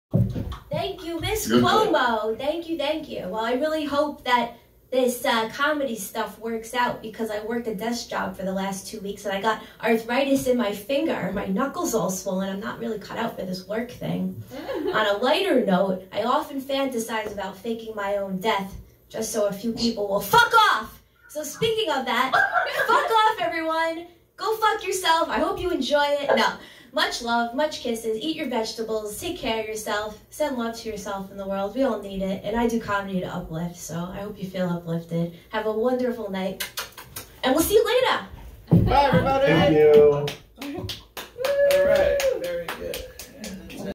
This is why I Keep Texts Short
1-word texts are common- pLease don't take short texts personally.

1. Texting & Gardening causes my finger to swell to the point where I can't do housework/tie shoes. The more I text, the more likely I am to disable myself.

2. Texting is a Time Suck that can Truly Ruin Lives. 5 mins of texting 30 people each day= 150 mins. 2 HOURS/day of correspondence (easy to hit 6) means we're not using 2 HOURS to engage in activities that can boost financial freedom, life fulfillment, & bring dreams into reality.  If you want to accomplish anything worthwhile, I Welcome you to send me 1 word texts. That shows me that you're Respecting your time & life quality.

Thanks for havin me host Mike Nicolia Governors Comedyclub & attending the final Zoom Shows of the Pandemic (hopefully!) Raymond Taliercio Sean Fennick Ronald Feldman